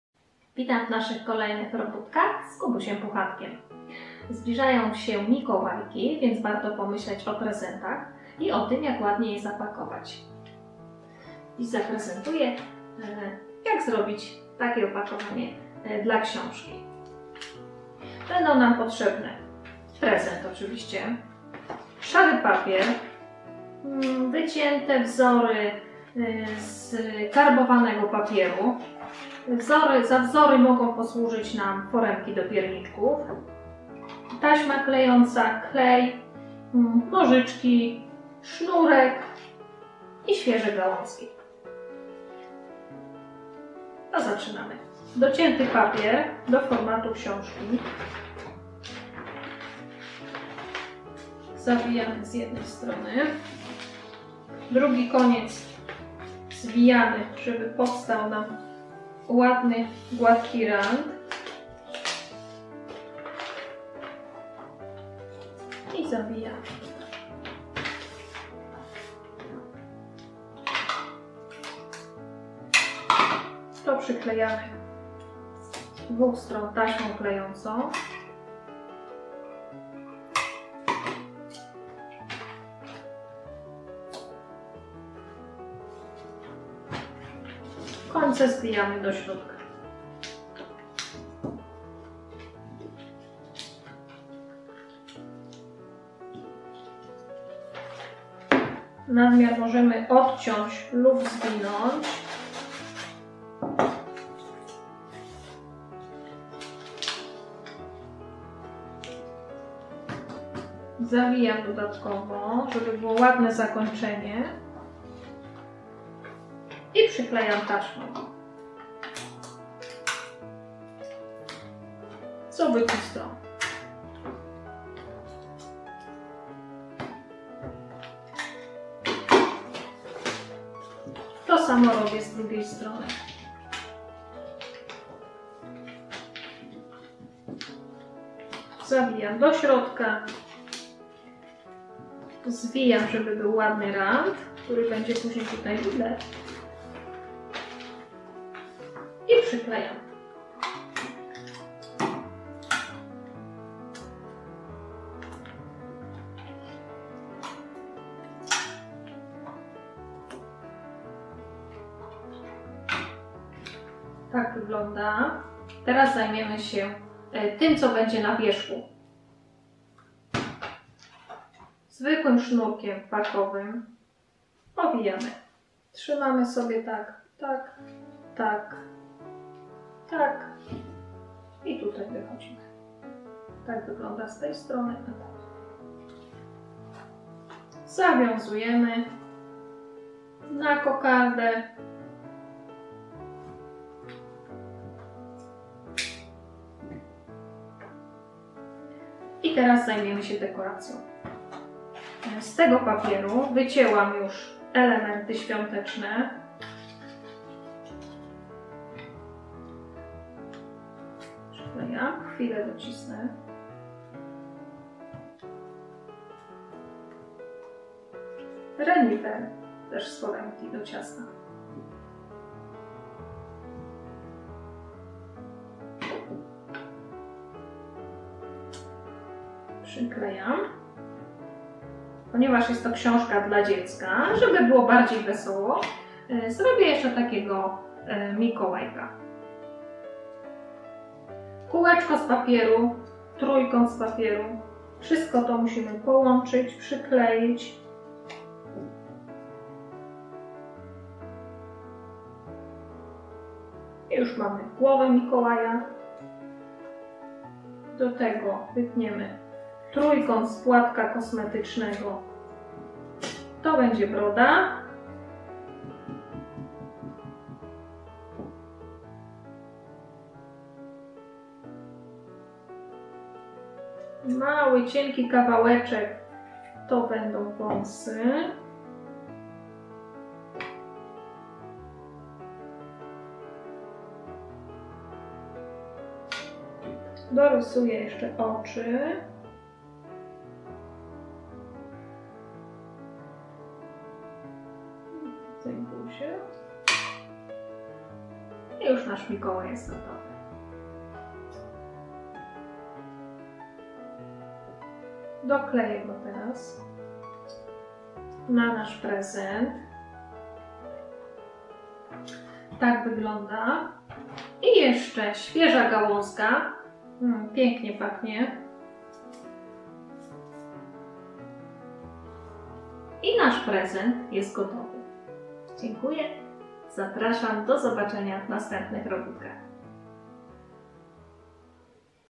Cztery pory roku z Kubusiem Puchatkiem - Mikołajkowy prezent
Mikołajki tuż, tuż ….
Pokażemy Wam, jak zapakować książkę.
Wiadomo przecież, że książka 📖 to najlepszy prezent 🎁 na świecie.